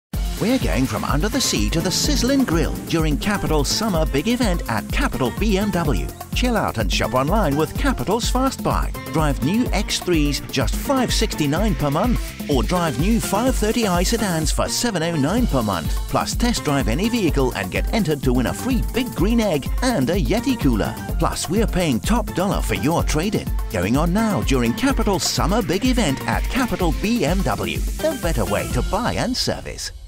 Capital BMW - Summer Big Event June
We’re going from under the sea to the sizzling grill! Test-drive any new BMW & get entered to win a Big Green Egg & Yeti Cooler during Capital's Summer Big Event! Don’t miss out - these incredible incentives won't last. Come experience why Capital is The Better Way to Buy & Service.
CAPITAL BMW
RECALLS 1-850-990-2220